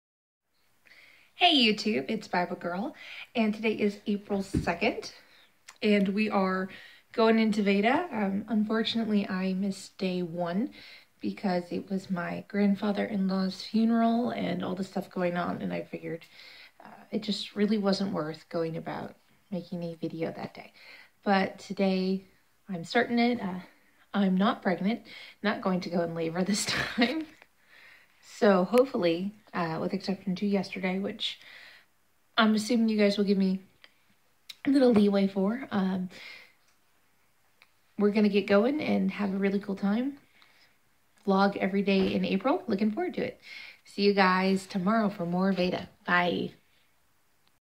VEDA begins (VEDA 19 # 1)
I missed yesterday due to a funeral but here's to the start of vlogging everyday in April